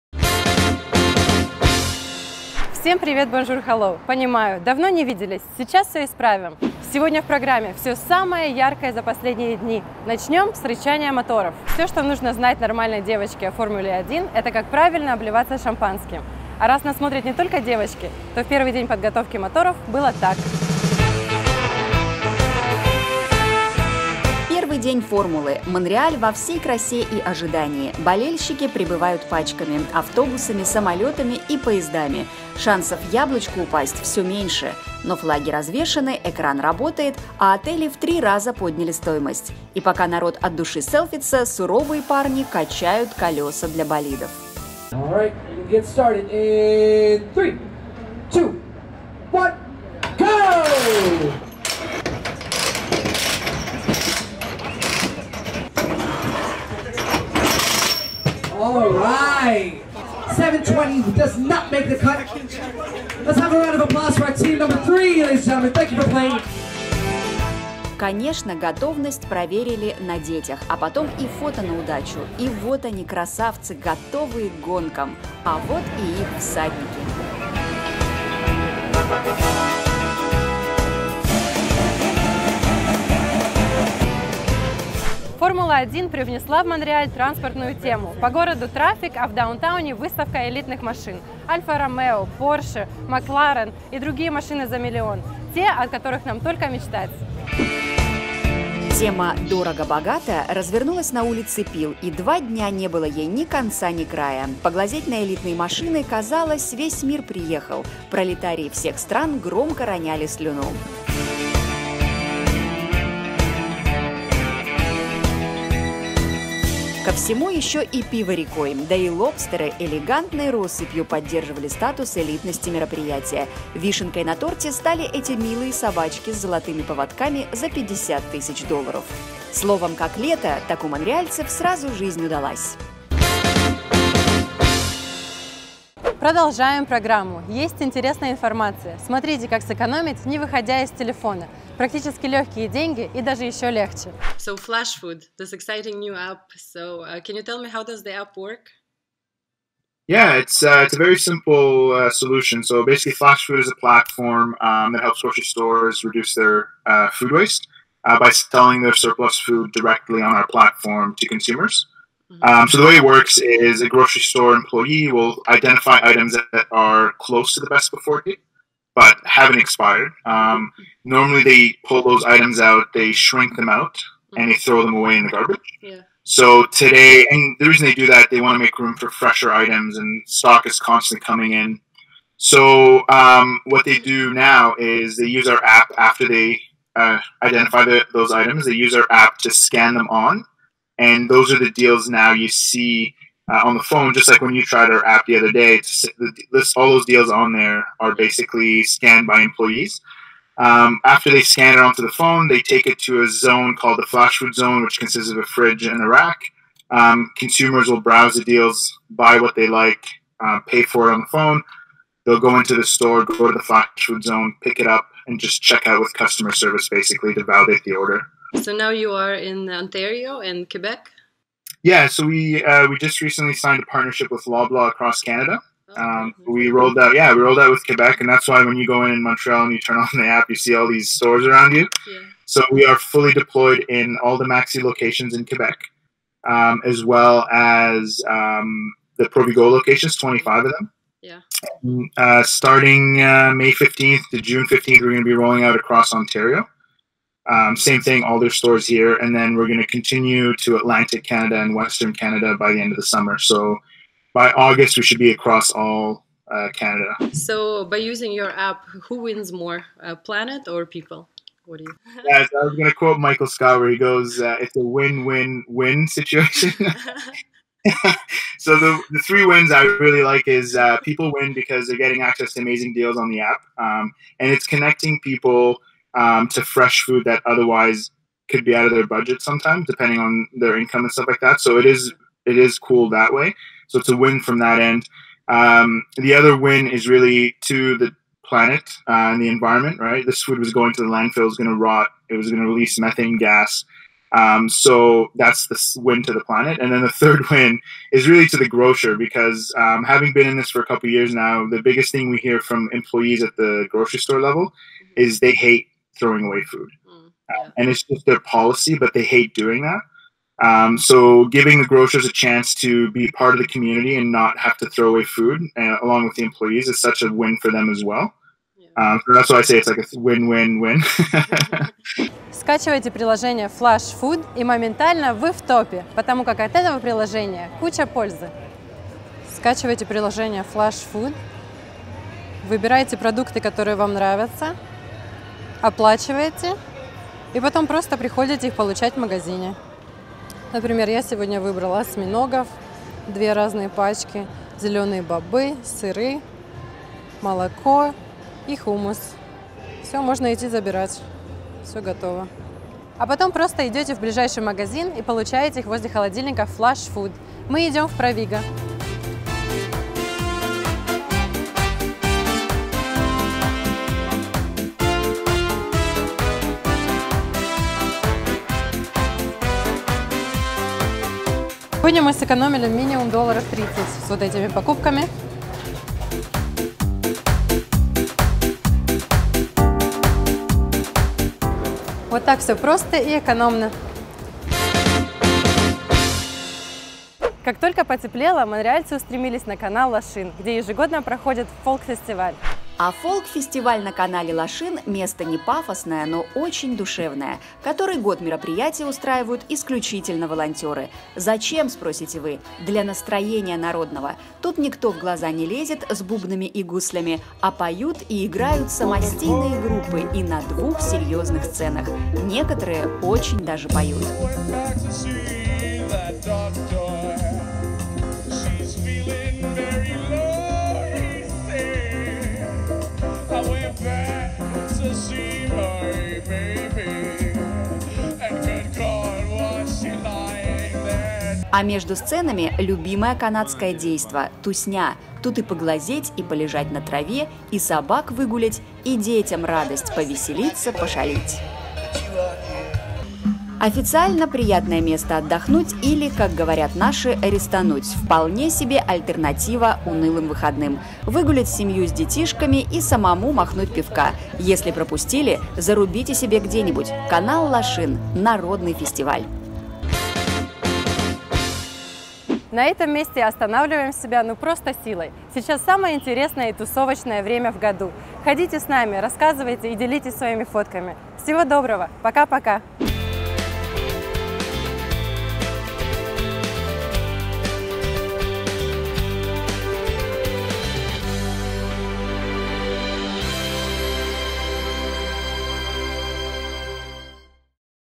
Folk Fest, Formula 1 and new app Flashfood Privet Bonjour Hello
Сегодня в программе - болиды, фестивали и все о приложении Flashfood, которое борется с проблемой пищевых отходов и помогает нам сэкономить одновременно.

privetbonjourhello.ca

Монреальская медиа компания, мы делаем нескучные местные новости.
Montreal based media production company, fun news focused on Russian community.